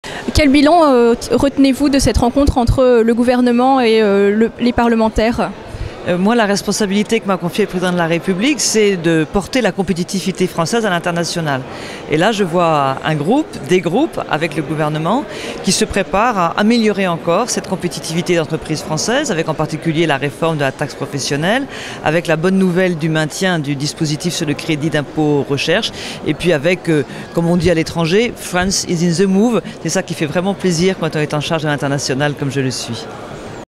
Anne-Marie Idrac: Bilan des Journées des Parlementaires 2009
Anne-Marie Idrac, Secrétaire d'Etat chargée du Commerce extérieur, auprès de la ministre de l'Economie, dresse son bilan personnel des Journées des Parlementaires 2009, organisées au Touquet. (24 septembre 2009)